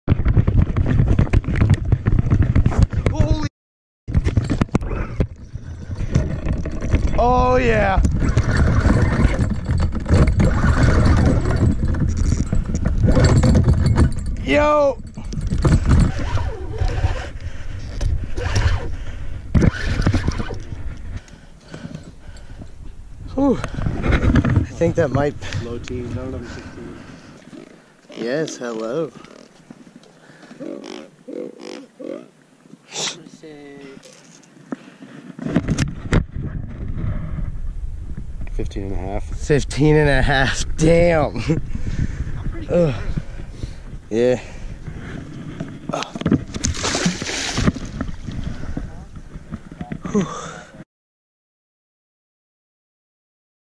BIG CATFISH almost TAKES ROD!
15 lbs Blue Catfish hits my rod HARD!!! ;)

PICTURE IN THUMBNAIL NOT MINE! CONTACT TO REMOVE